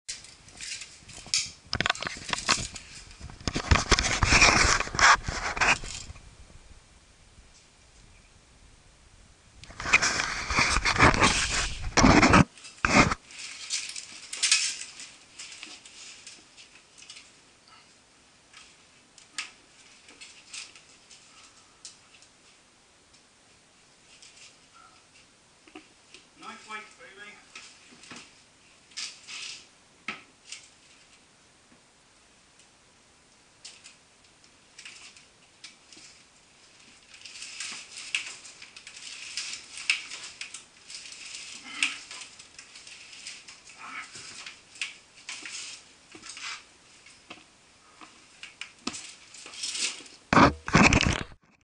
Knight Weight Baby (Armoured Chinups)!
First time attempting chinups in armour. Next time I'll wear the leg and and hand armour too.

Currently sitting around 220lbs after losing a lot of weight from illness.

Armour weight 54lbs